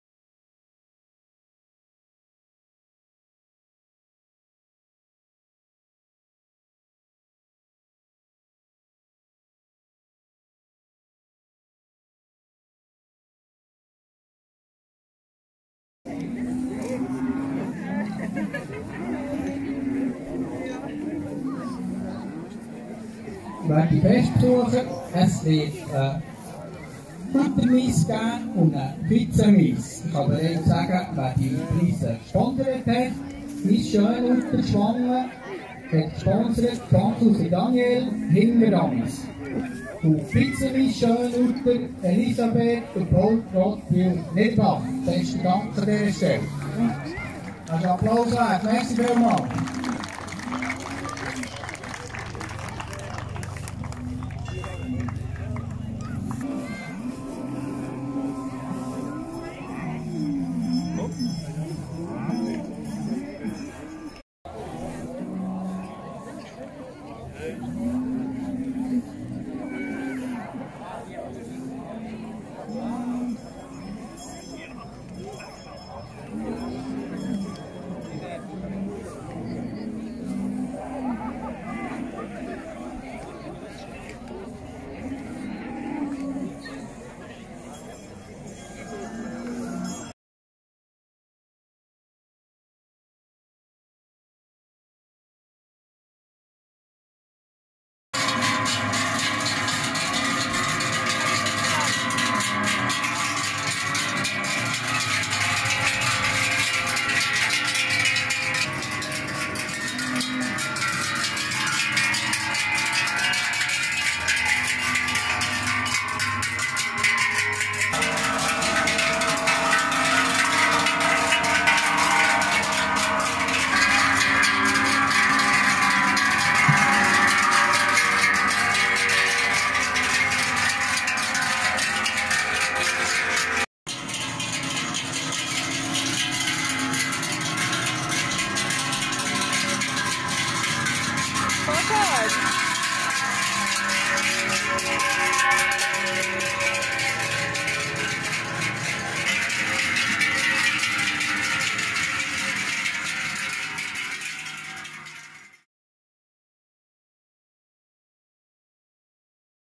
Die Jubiläumsschau des Viehzuchtvereins Schwanden im Emmental 2021
In Schwanden im Emmental konnte am 2. Oktober 2021 endlich gefeiert werden. Die Mitglieder des Viehzuchtvereins Schwanden führten rund 300 Kühe auf.

Interessiert an mehr Inhalten wie diesem? Hier geht es zu unseren Abos